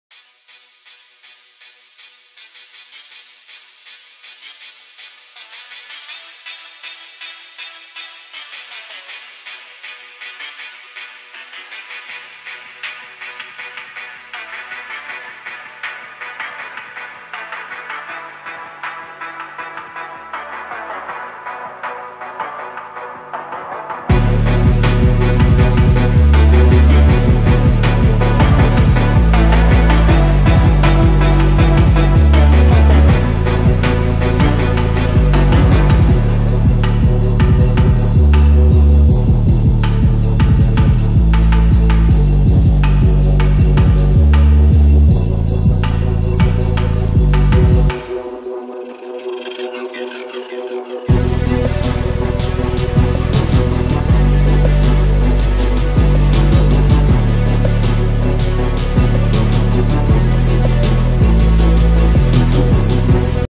Bearded Johnny Bravo Speedart in Procreate (iPad Pro Art)
Drawing Bearded Johnny Bravo on iPad Pro.

**Follow me**

Tools used:

iPad Pro 12.9 in

Apple Pencil

Procreate